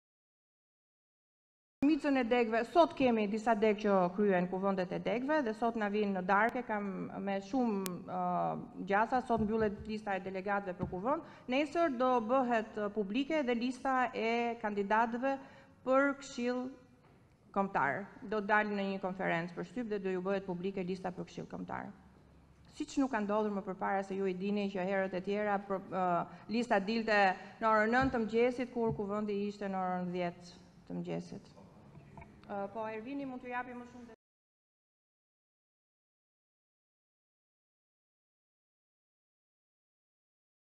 Vokshi: Nesër del lista e kandidatëve për Këshill Kombëtar, s’ka ndodhur kurrë
Deputetja e Partisë Demokratike, Albana Vokshi deklaroi ditën e sotme se nëse do të bëhet publike lista e Kandidatëve për Këshillin Kombëtar. Kjo gjë nuk ka ndodhur më parë, tha Vokshi.